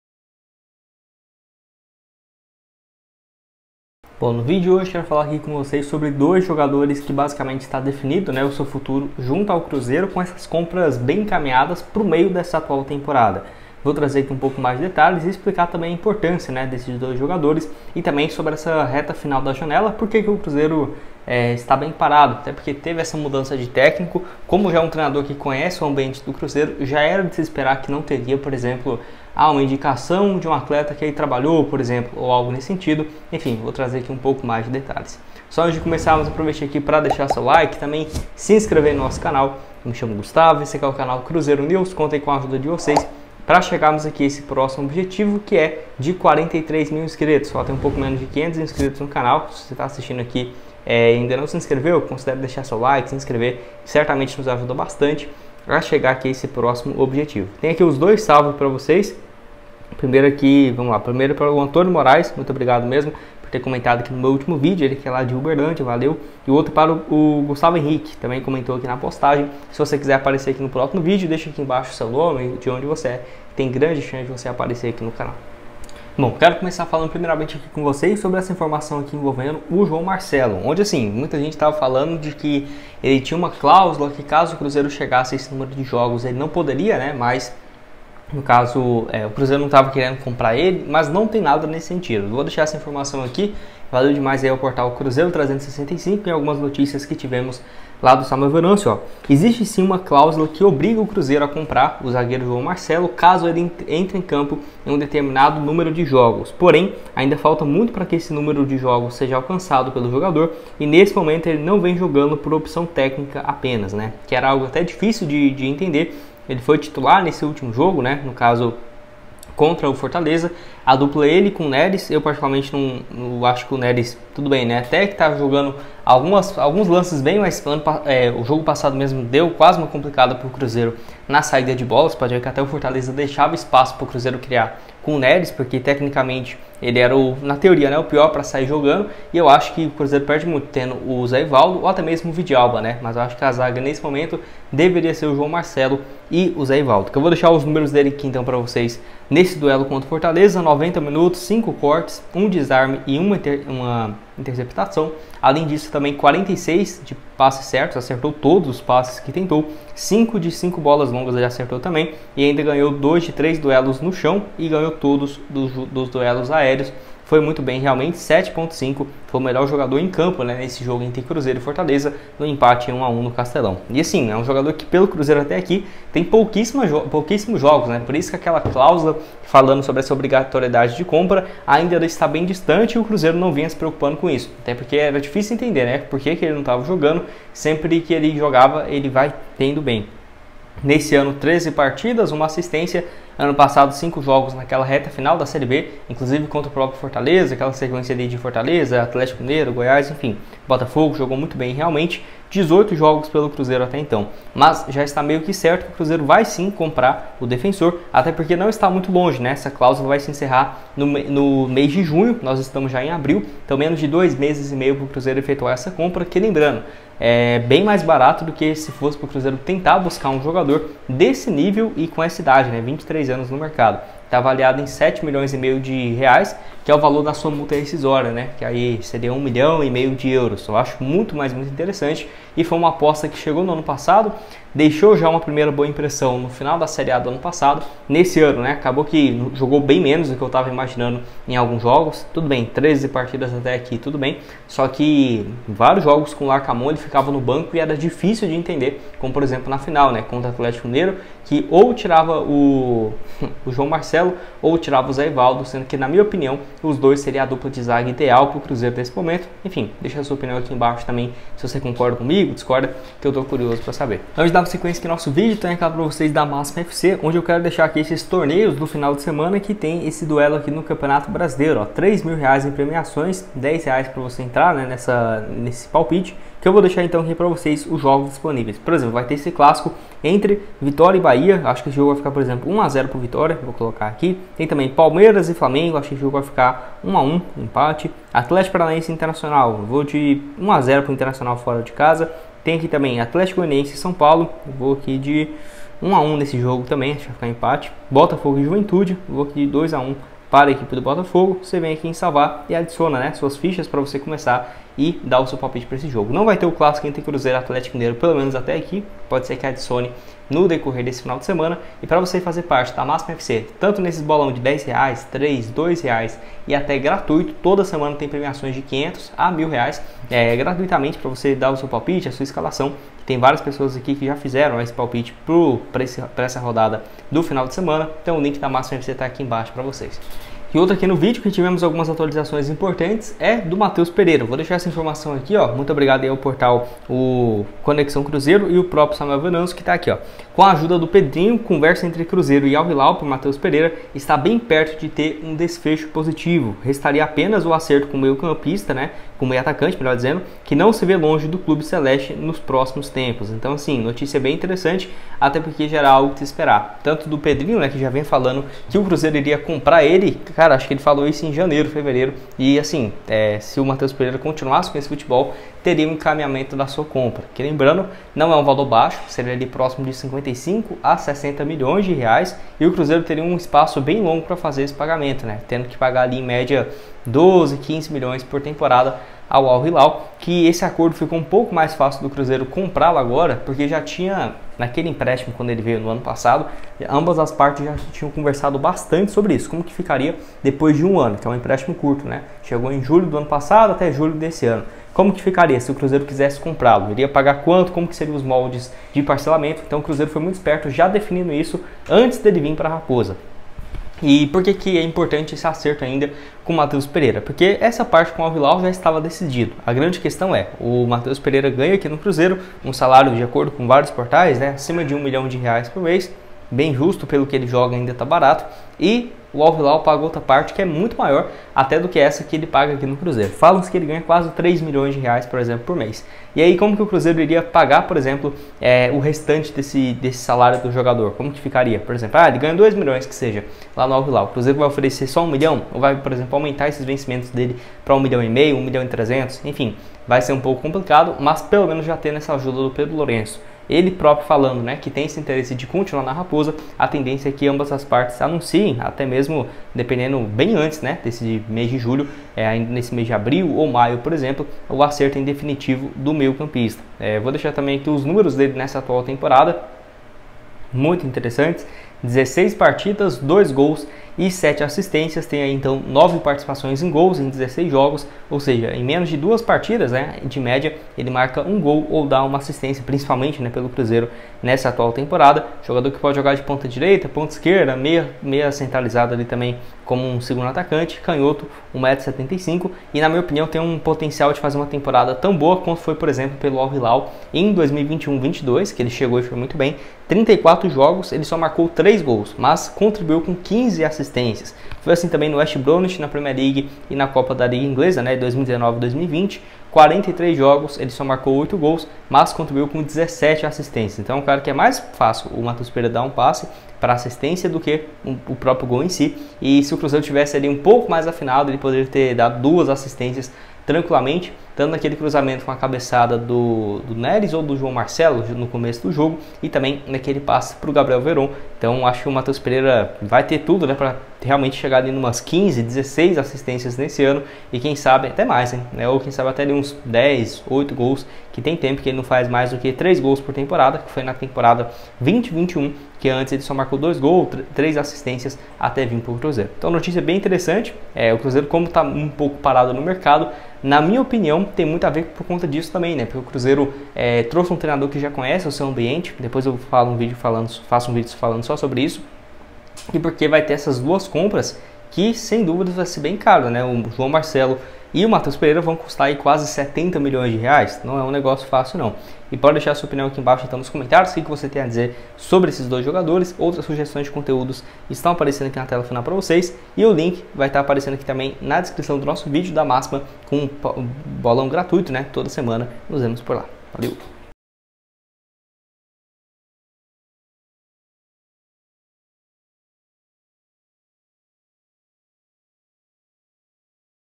✅🔥BOMBA: 2 REFORÇOS COMPRADOS PELO CRUZEIRO PRA PRÓXIMA JANELA! E MAIS
⚽️✅SEU PALPITE GRÁTIS NA MÁXIMA FC COM PREMIAÇÕES DE R$;300 . REGISTRE-se

🔥Meu perfil no Instagram 👇@gustavo.machado.21 e @cruzeironews21_